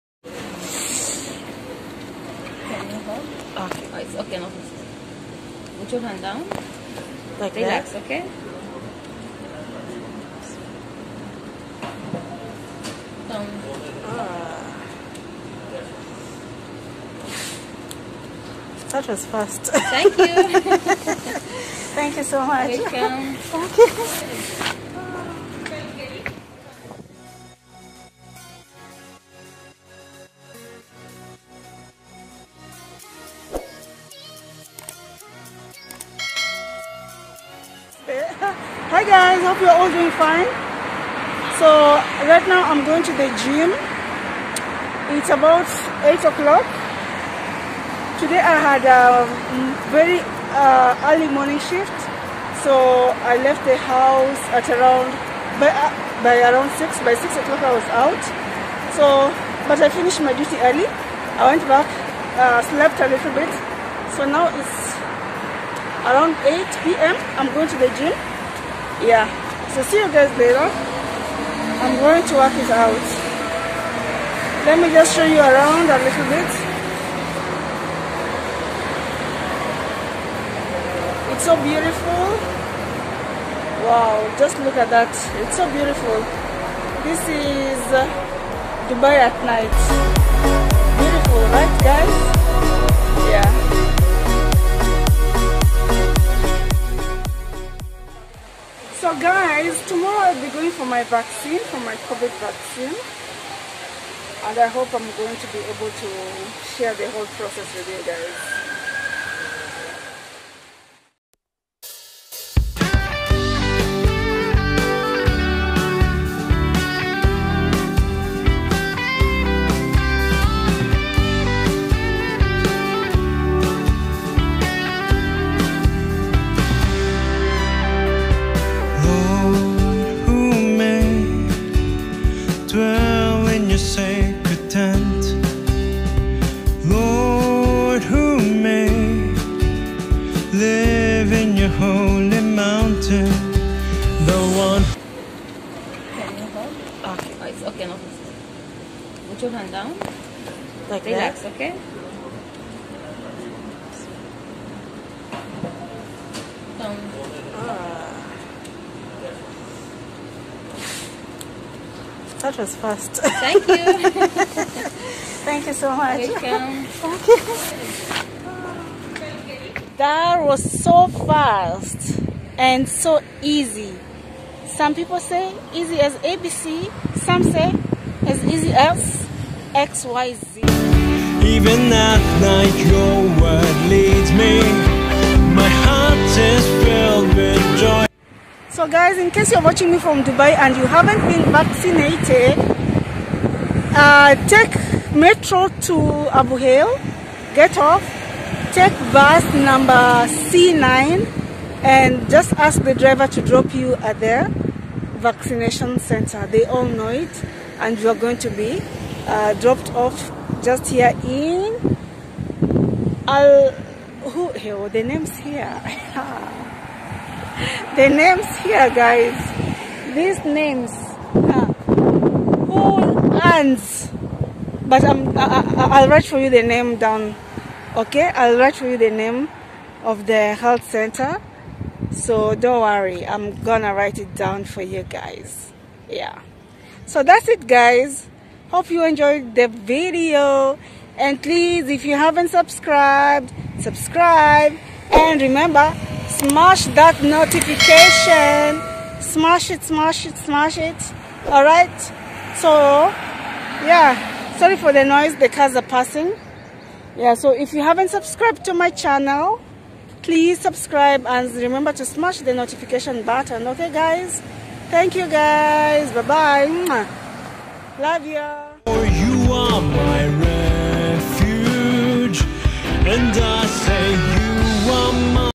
Covid 19 Vaccinated | All Things Lillian
Hi guys, I'm finally vaccinated!

kindly continue to mask up and take care of yourself and loved ones.